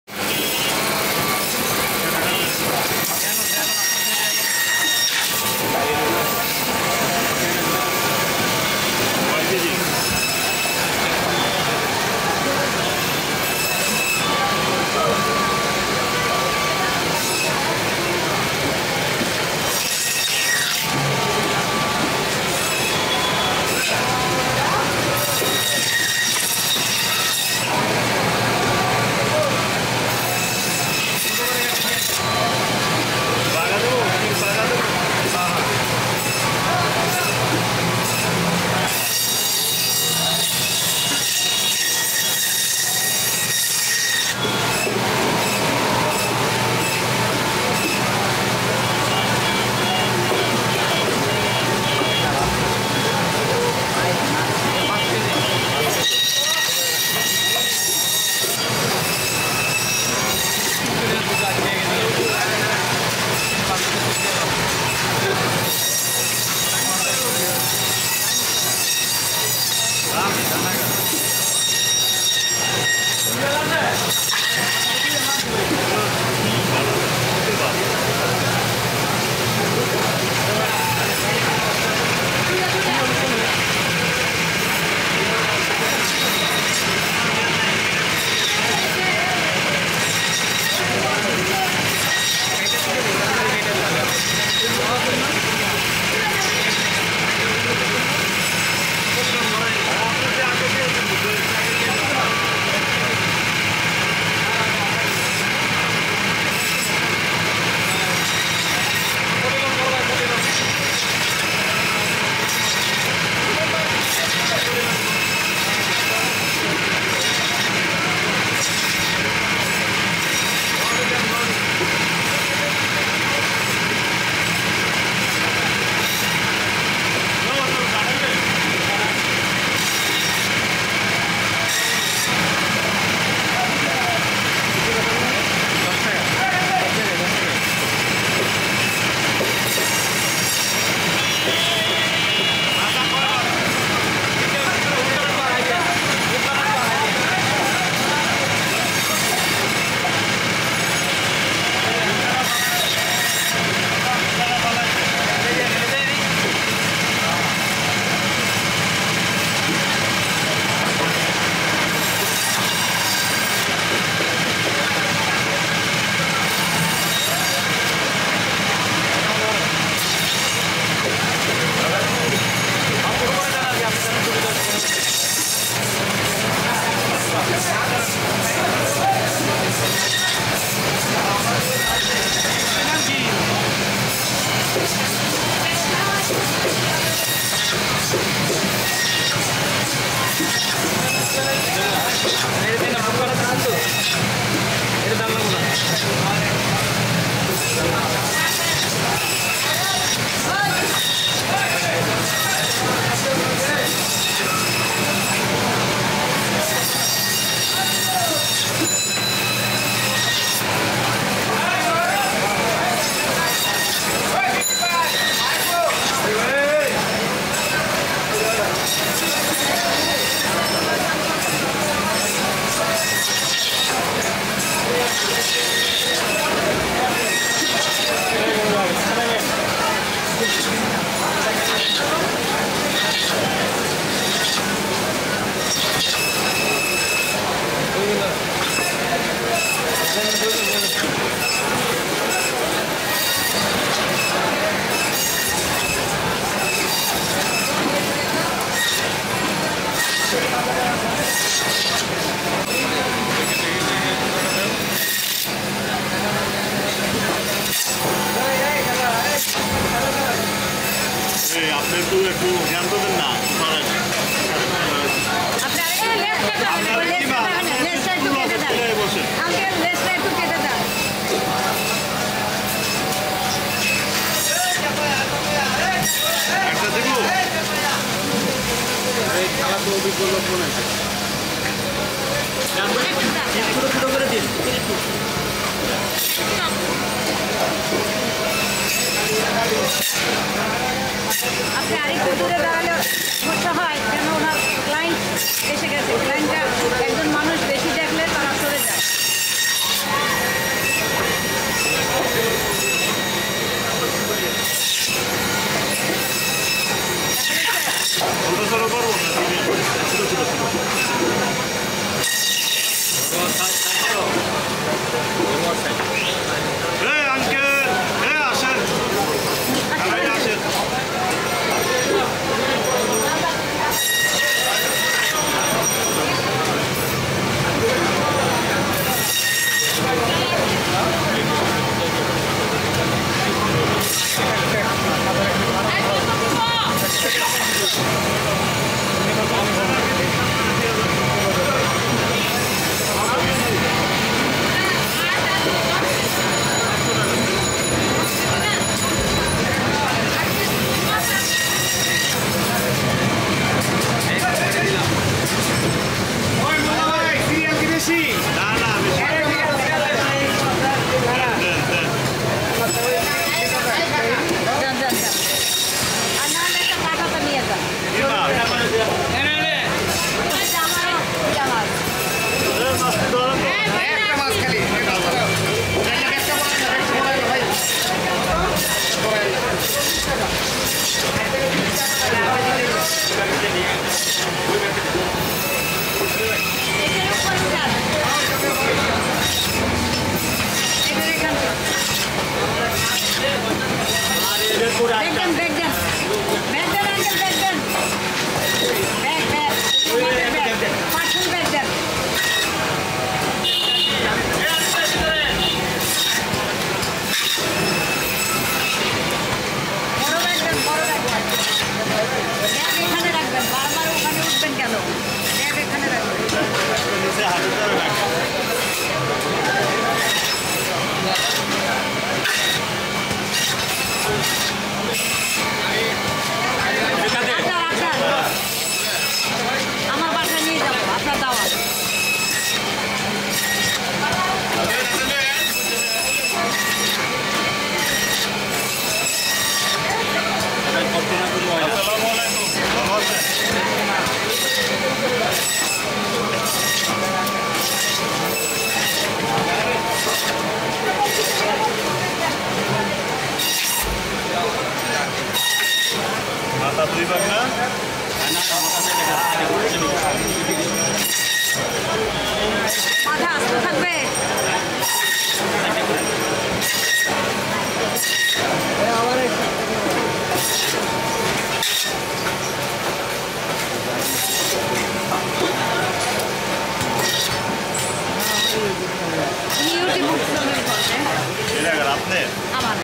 Cutting Of Large Fish By Machine | Giant Rohu Fish Cleaning & Chopping By Expert Fish Cutter
mazing Cutting Skills | Big Rohu Fish Skinning & Chopping By Expert Fish Cutter | Fish Cutting | Fish